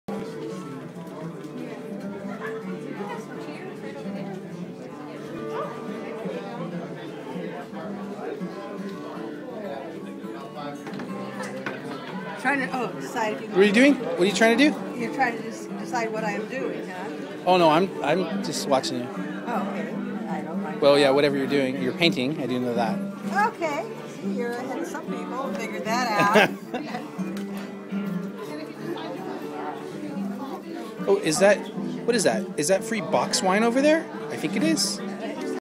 Artist painting at First Friday Bakersfield, CA
Sorry, I didn't get the artist's name but her work is lovely.